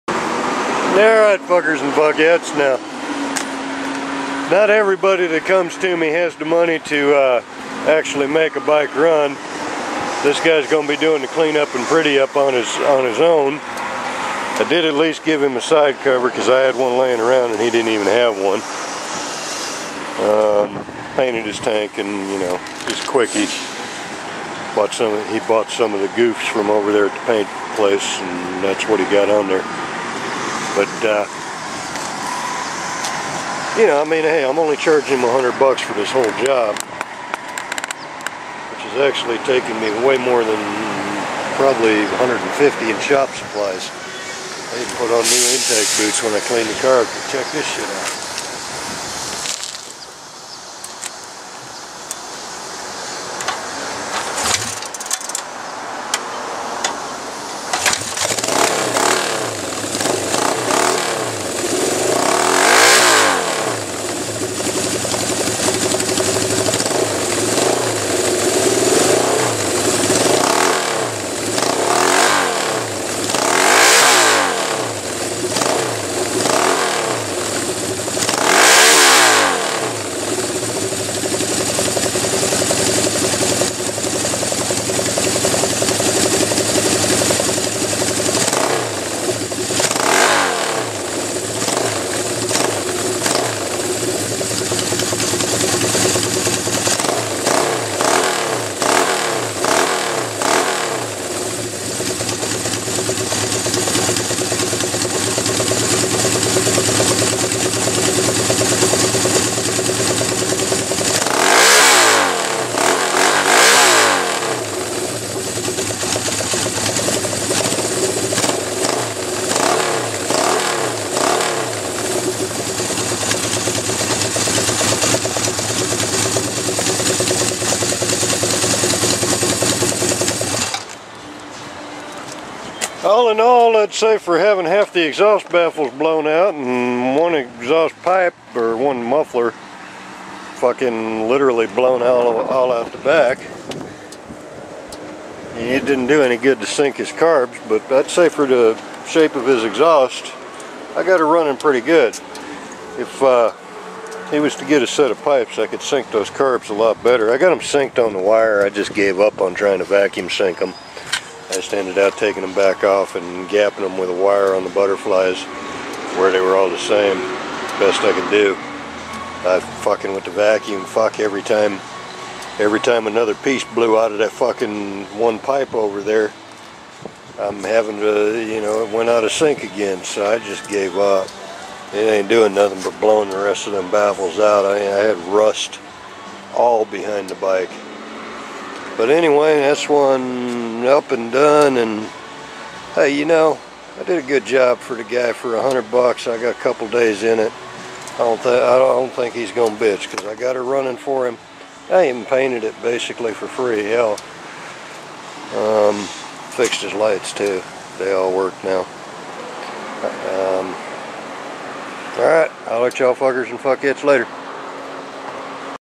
1972 Honda CB750 - up and done
Dudes doing the rest of the pretty up on his own -- This was just a quickie for a friend